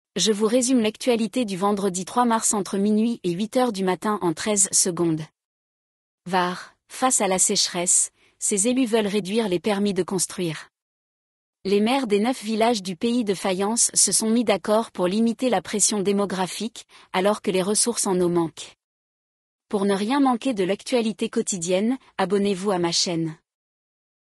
Résumé de l'actualité du jour : Vendredi 03 mars entre minuit et 8h
- Var : face à la sécheresse, ces élus veulent réduire les permis de construire
- L'Assemblée vote l'allongement du congé pour décès d'un enfant
- Canada : un enfant de 4 ans blesse par balle un enfant de 6 ans
- Météo : un vendredi gris en perspective
- Messages et « chantage » sexuels : le profil trouble de Daniel G., ex-patron des arbitres d’Île-de-France
- Film porno en cours, flirt avec des apprentis… Un formateur renvoyé de la fac des métiers de l’Essonne
- Vaires-sur-Marne : devant les assises pour avoir porté 19 coups de couteau à sa belle-fille
- Île-de-France : à 80 ans, Marc blanchissait l’argent des faux techniciens Windows
- Un ami de Leslie et Kevin mis en examen, Macron en road trip africain, dernier jour pour sauver la haute mer… L’actu de ce vendredi matin
- Accident de trains en Grèce : le deuil et la colère s’emparent du pays
- Canada : un enfant de 6 ans blessé par balle par un autre enfant, âgé de 4 ans